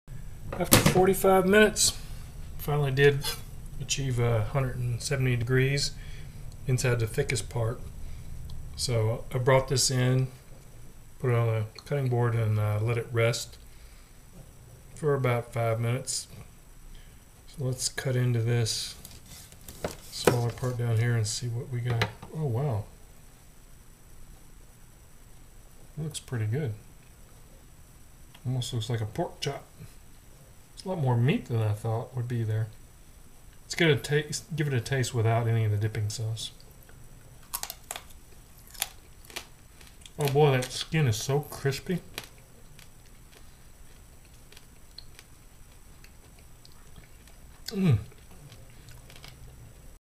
Asian-Style Pork Belly Teaser
Grilled some Asian inspired pork belly today. It was very tasty, shot some video but it's almost 15 mins long so I thought I'd upload a little teaser video first. inkidreferralcode tomstestkitchen